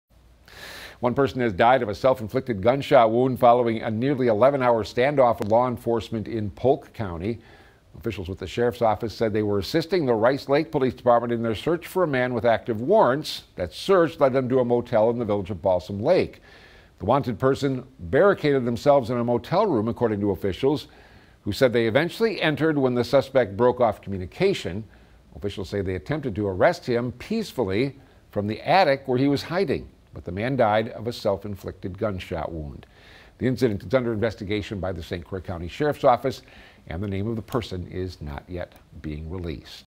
Man dead after over 11-hour standoff with police in Balsam Lake
One person has died of a self-inflicted gun shot wound following a nearly 11-hour standoff with law enforcement, according to the Polk County Sheriff's Office.